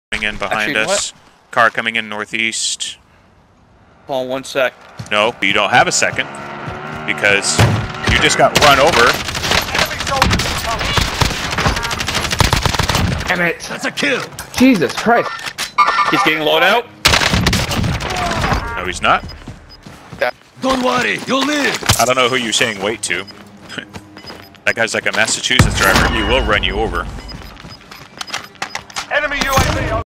You don't HAVE a second!!
Teammate (Twitch.TV/AgentUseless) tried to grab loadout and needed 1 more second!